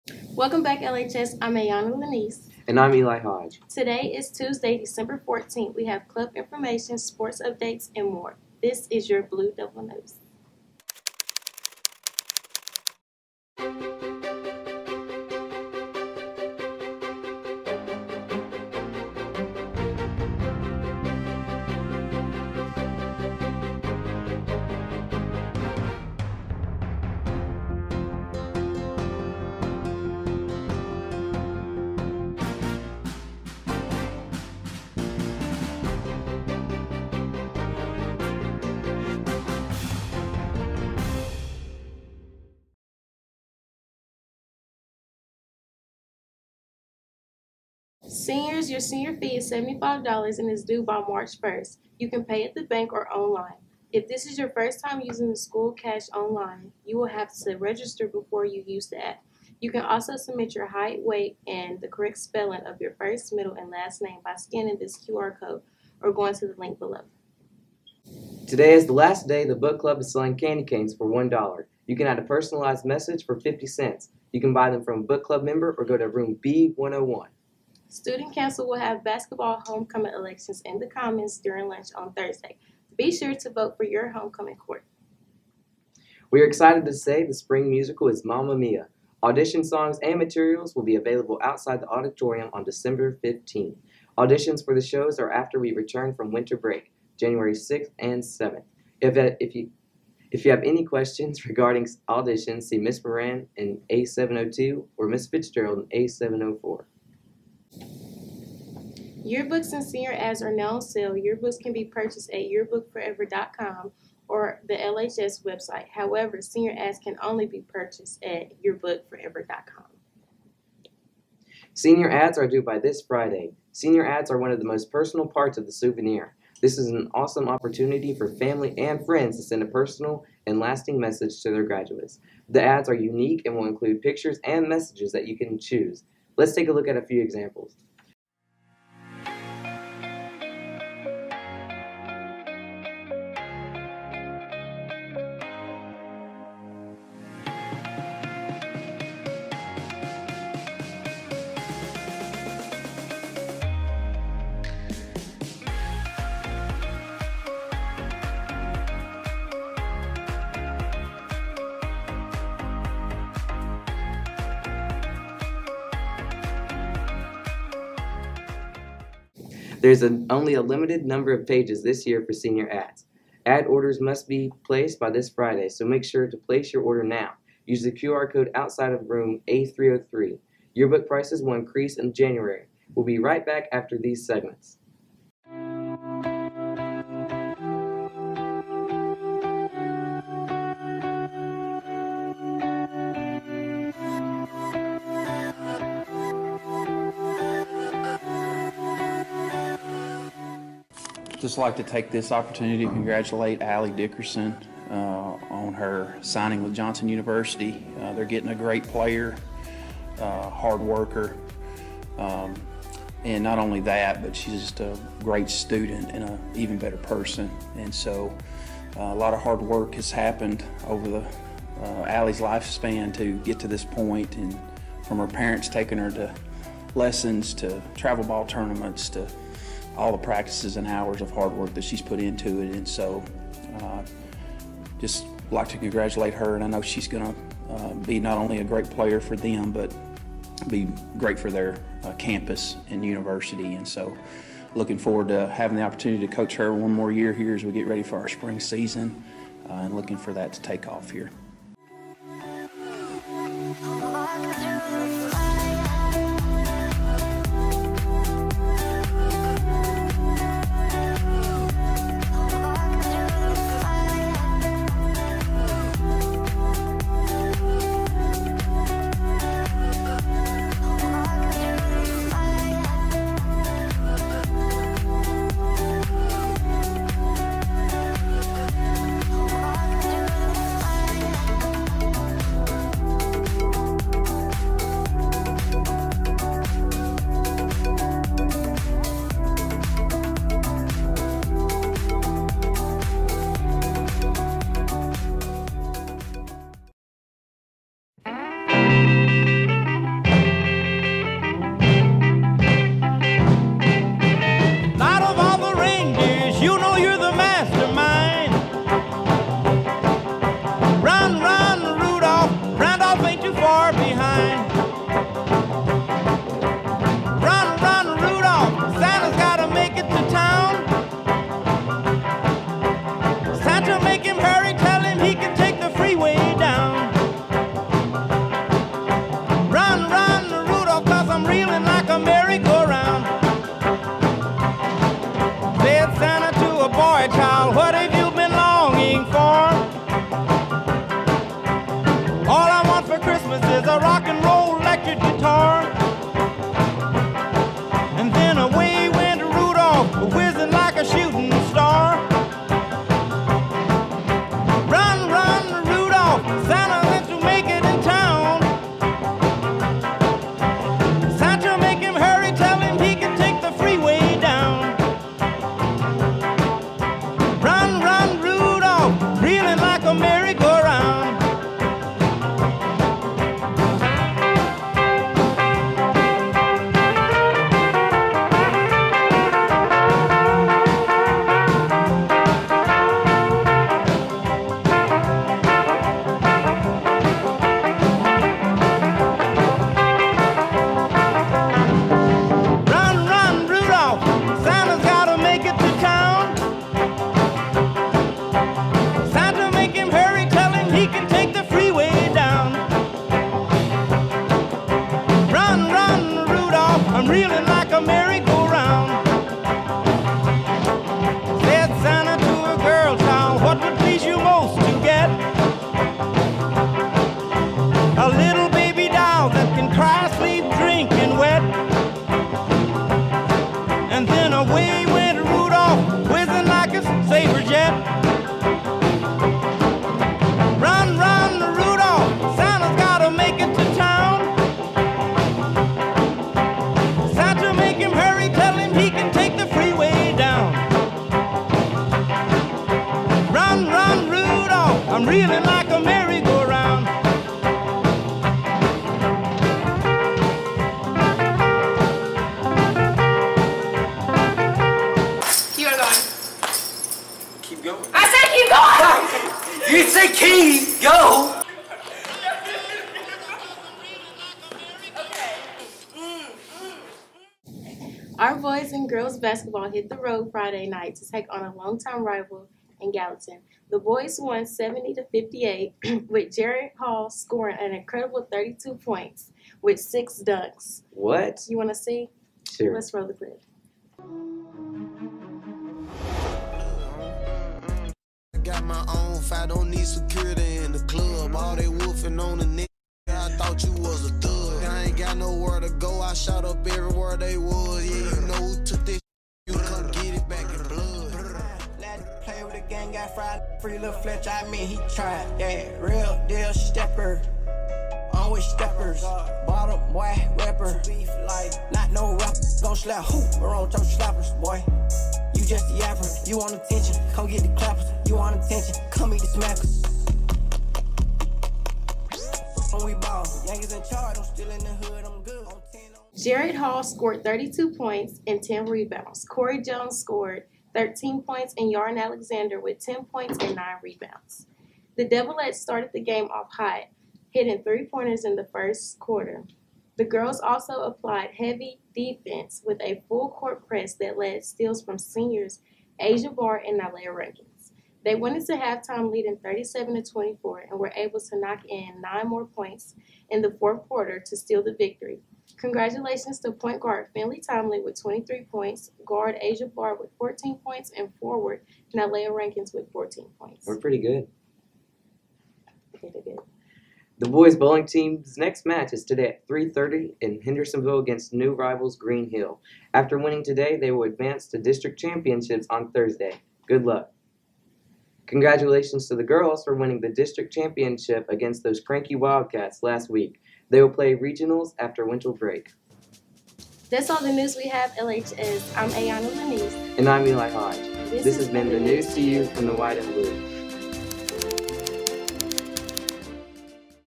LHS BDN - December 14, 2021
LHS BDN Student News - Lebanon High School Blue Devil News - Student News December 14, 2021 with Ayunah Sneed and Eli Hodge

00:00
00:47 General Info
02:17 SR Ad Examples
03:05 Aly Dickerson Signing
04:59 AV1 Students Holiday Lip Sync
08:06 Jared Hall Dunks
09:53 Outro